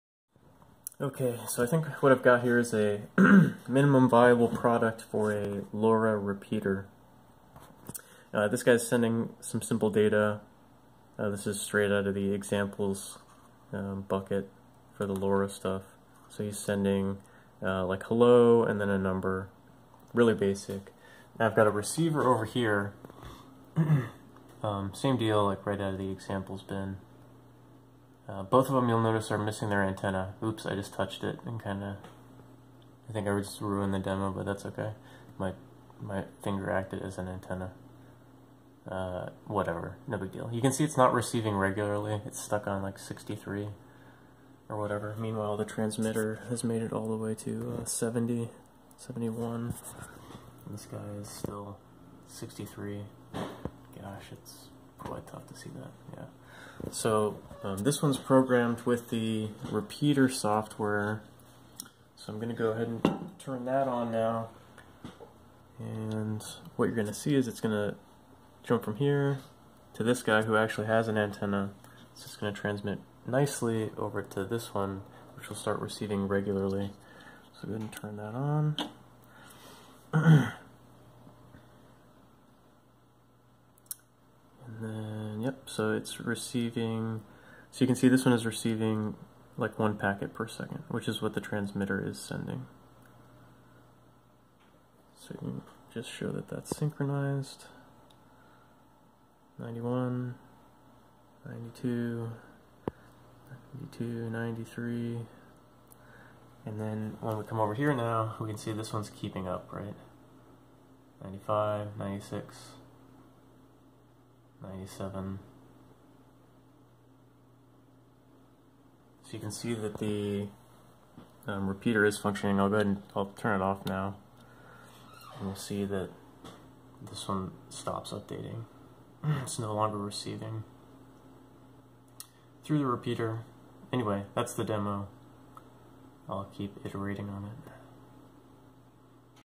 LoRa repeater
Simple LoRa Repeater as a POC for defeating RSSI based CactusCoin challenge.